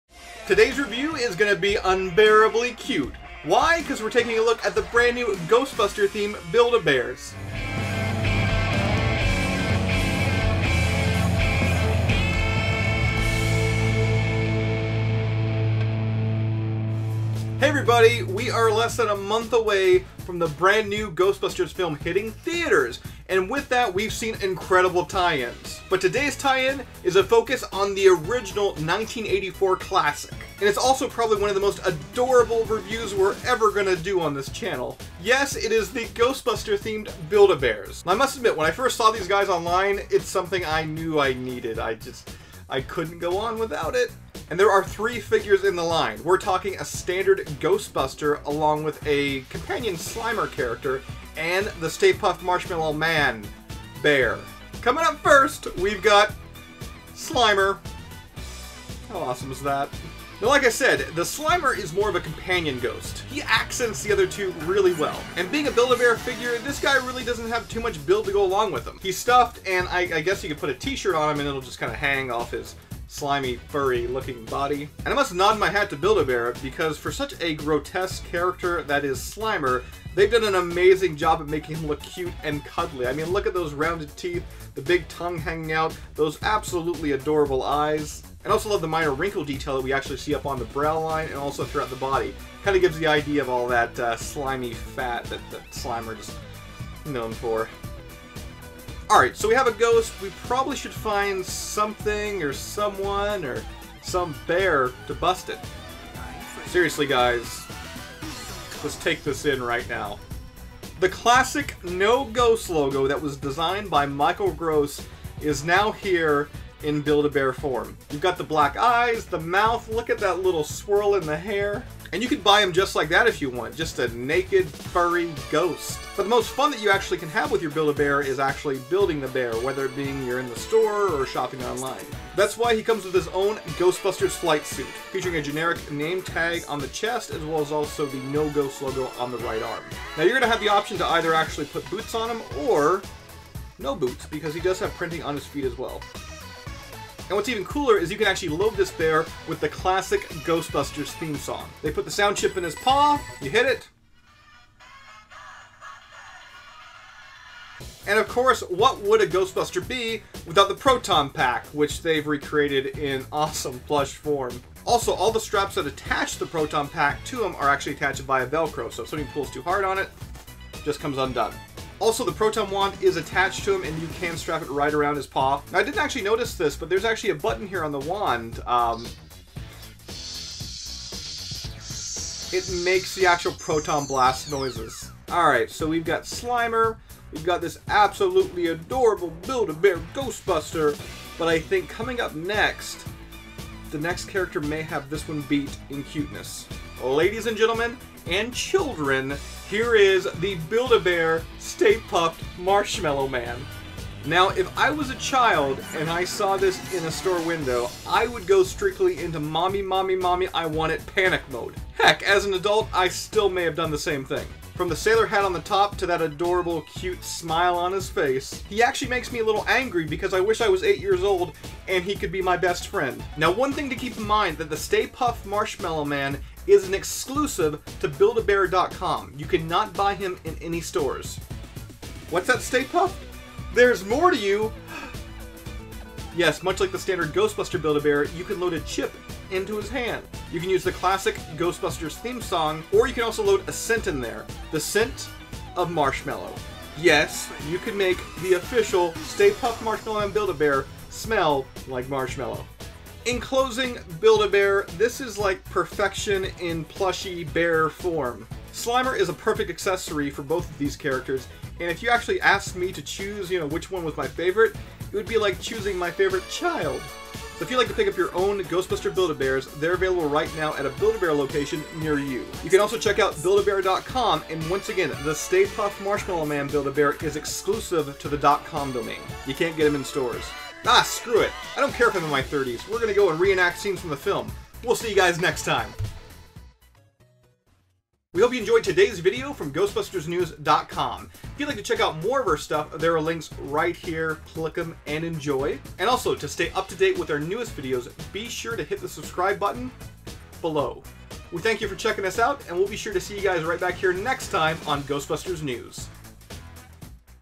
Ghostbusters Build-A-Bear Unboxing + Review
Today we take a look at the brand new Ghostbusters themed Build-A-Bear bears!